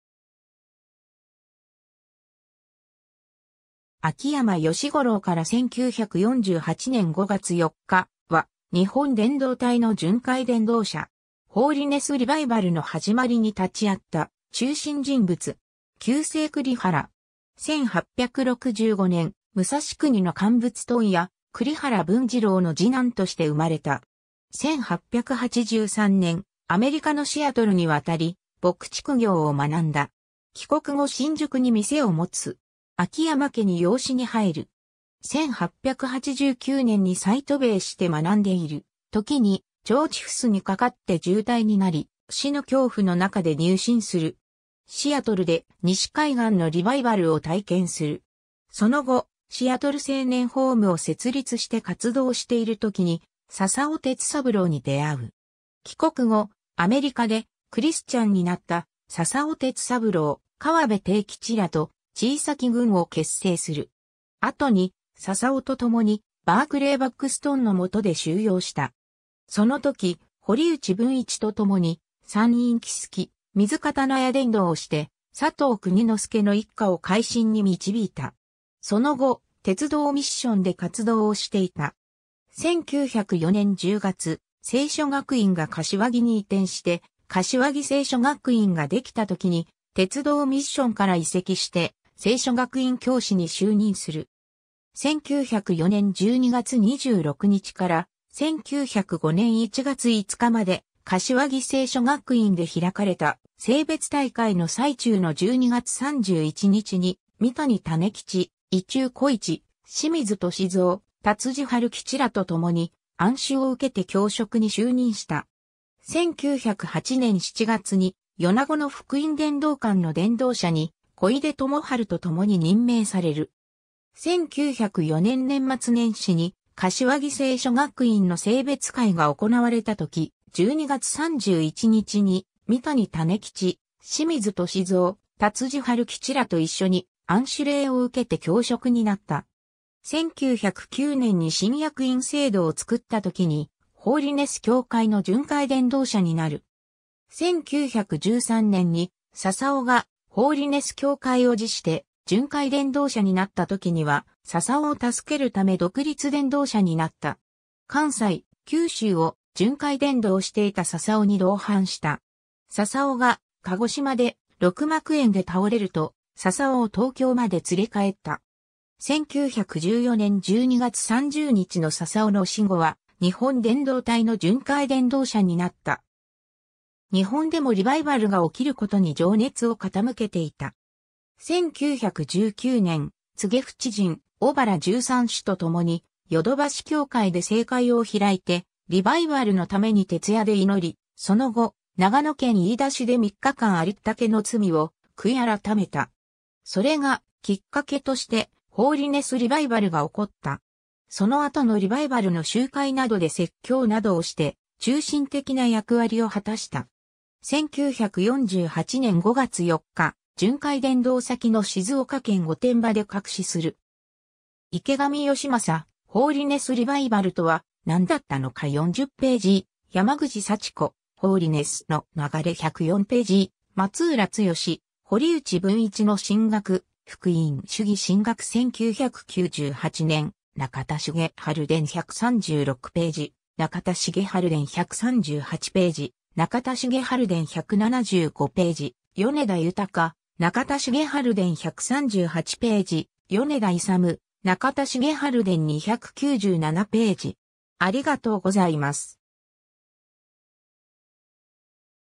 秋山由五郎
秋山 由五郎（あきやま よしごろう、1865年8月5日（慶応元年6月14日） - 1948年（昭和23年）5月4日）は、日本伝道隊の巡回伝道者。
ホーリネス・リバイバルの始まりに立会った中心人物。
旧姓栗原。
 1865年、武蔵国の乾物問屋栗原文治郎の次男として生まれた。
1883年、アメリカのシアトルに渡り牧畜業を学んだ。
帰国後新宿に店を持つ。
秋山家に養子に入る。
1889年に再渡米して学んでいる時に、腸チフスに罹って重態になり、死の恐怖の中で入信する。
シアトルで西海岸のリバイバルを体験する。
その後、シアトル青年ホームを設立して活動している時に笹尾鉄三郎に出会う。
帰国後、アメリカでクリスチャンになった笹尾鉄三郎、河辺貞吉らと「ちいさき群」を結成する。
後に、笹尾と共に、バークレー・バックストンの下で修養した。
その時、堀内文一と共に、山陰木次、水刀屋伝道をして佐藤邦之助の一家を回心に導いた。
 その後、鉄道ミッションで活動をしていた。
 1904年10月、聖書学院が柏木に移転して、柏木聖書学院ができたときに、鉄道ミッションから移籍して聖書学院教師に就任する。
1904年12月26日から、1905年1月5日まで柏木聖書学院で開かれた、聖別大会の最中の12月31日に、三谷種吉、伊中小市、清水俊蔵、多辻春吉らと共に按手を受けて教職に就任した。
1908年7月に米子の福音伝道館の伝道者に、小出朋治と共に任命される。
1904年年末年始に柏木聖書学院の聖別会が行われた時、12月31日に、三谷種吉、清水俊蔵、多辻春吉らと一緒に按手礼をうけて教職になった。
1909年に新役員制度を作った時に、ホーリネス教会の巡回伝道者になる。
1913年に笹尾がホーリネス教会を辞して巡回伝道者になった時には、笹尾を助けるため独立伝道者になった。
関西、九州を巡回伝道していた笹尾に同伴した。
笹尾が鹿児島で肋膜炎で倒れると、笹尾を東京まで連れ帰った。
1914年12月30日の笹尾の死後は、日本伝道隊の巡回伝道者になった。
日本でもリバイバルが起きることに情熱を傾けていた。
 1919年、柘植不知人、小原十三司と共に淀橋教会で聖会を開いて、リバイバルのために徹夜で祈り、その後、長野県飯田市で3日間ありったけの罪を悔い改めた。
それがきっかけとしてホーリネス・リバイバルが起こった。
その後のリバイバルの集会などで説教などをして中心的な役割を果たした。
 1948年5月4日、巡回伝道先の静岡県御殿場で客死する。
 ^ 池上良正『ホーリネス・リバイバルとは何だったのか』40ページ ^ 山口幸子『ホーリネスの流れ』104ページ ^ 松浦剛「堀内文一の神学」『福音主義神学（29号）』1998年 ^ 『中田重治伝』136ページ ^ 『中田重治伝』138ページ ^ 『中田重治伝』175ページ ^ 米田豊『中田重治伝』138ページ ^ 米田勇『中田重治伝』297ページ